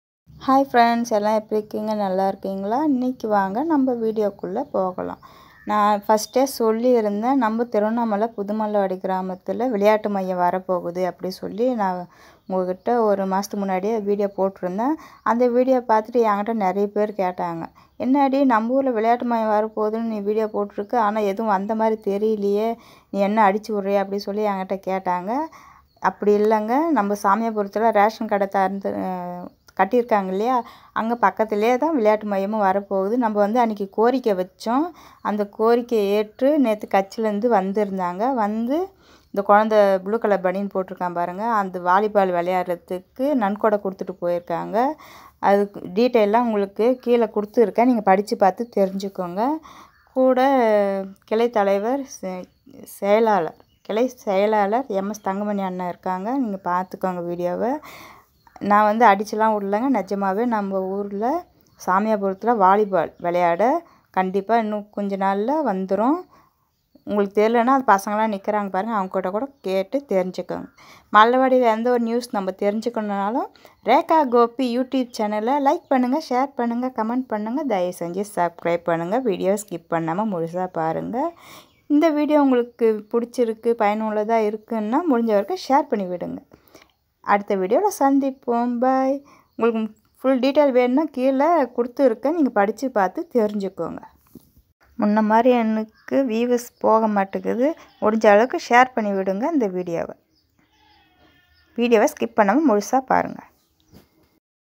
விளையாட்டு# மையம்மா கால் பந்து விளையாட்டா இது பொய்யாஉண்மையா
நான் சார்ந்த புதுமல்லவாடி கிராமத்தில் நாம் விடுத்திருந்த கோரிக்கையை ஏற்று நமது இளைஞர்களுக்காக நமது கிராமத்தில் கட்டப்பட்டுள்ள புதிய ரேஷன் கடை அருகில் நிரந்தர கைபந்து(volley ball) மைதானம் அமைக்கப்பட்டது…மாண்புமிகு சட்டப்பேரவை துணை சபாநாயக குபிச்சாண்டி அண்ணன் அவர்களுக்கும், மதிப்பிற்குரிய இளைஞரணி அமைப்பாளர் அண்ணன் CNஅண்ணாதுரை MP அவர்களுக்கும்,கிளை கழக செயளாலர் MSதங்கமணி அவர்களுக்கும் நன்றியை தெரிவித்துக் கொண்டு...
இன்று காலையில் தினம் துரிஞ்சாபுரம் மேற்கு ஒன்றிய செயலாளர் பாரதிராமஜெயம் அவர்கள் நமது விளையாட்டு அரங்கத்தை பார்வையிட்டு நன்கொடை வழங்கினார் இளைஞர்கள் சார்பாக அவர்களுக்கும் நன்றியை தெரிவித்துக் கொள்கிறோம் 🙏🏼